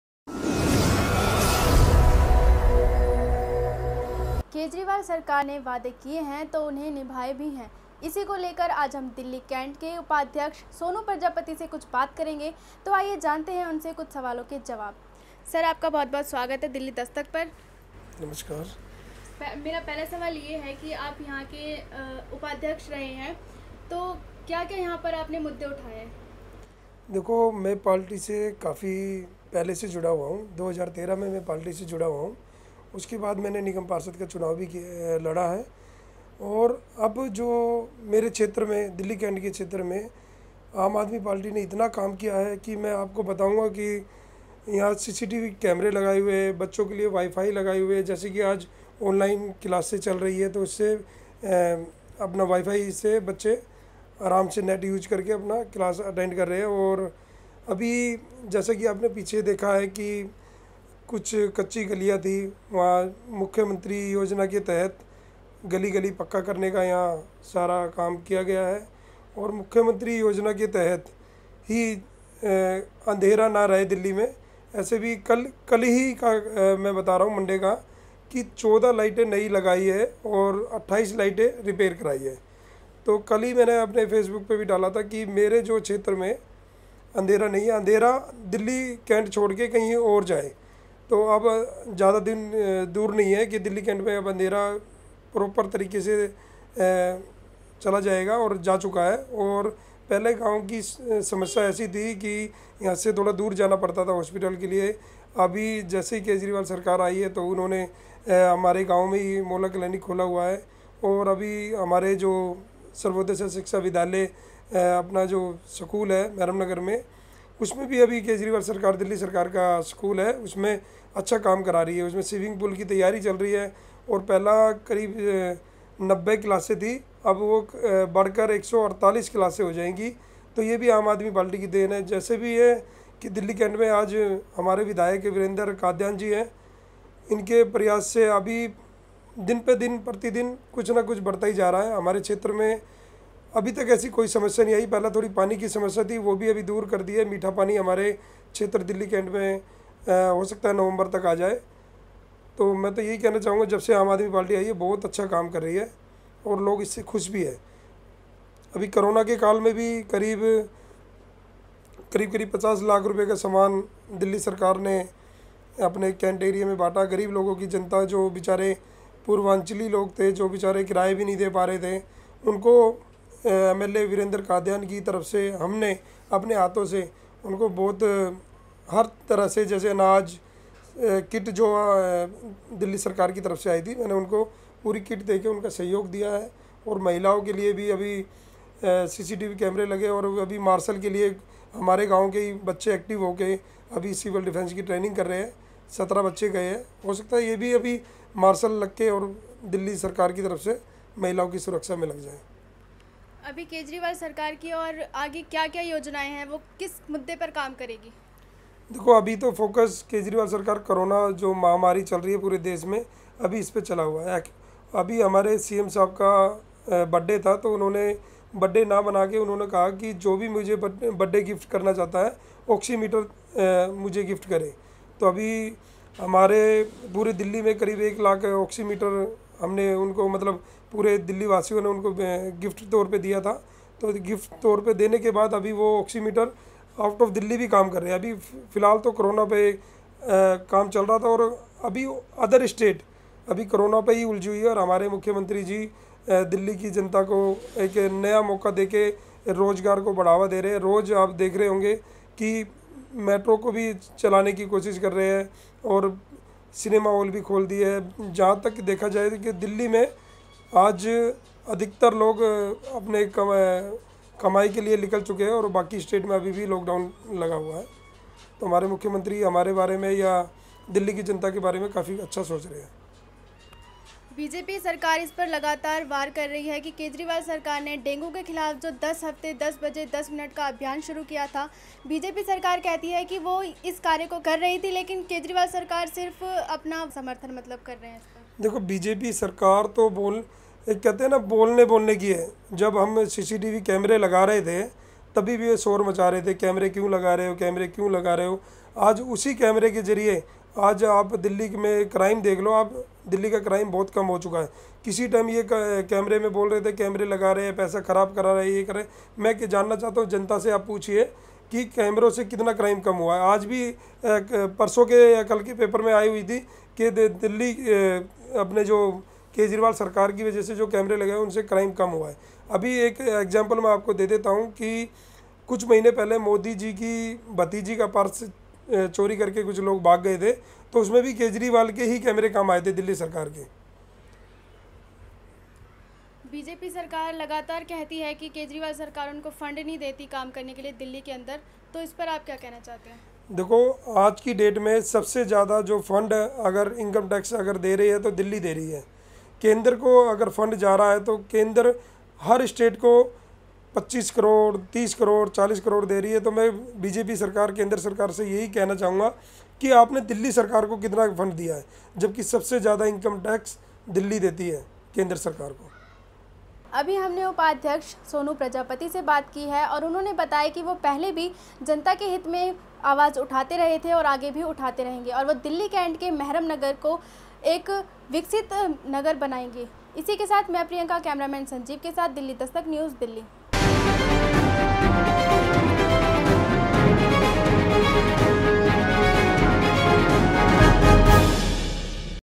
आप पार्टी के दिल्ली केंट के उपाध्यक्ष ने गिनवाए केंट में किये गए पार्टी के कार्य
Hi

About This Video :- आप पार्टी के दिल्ली केंट के उपाध्यक्ष ने गिनवाए केंट में किये गए पार्टी के कार्य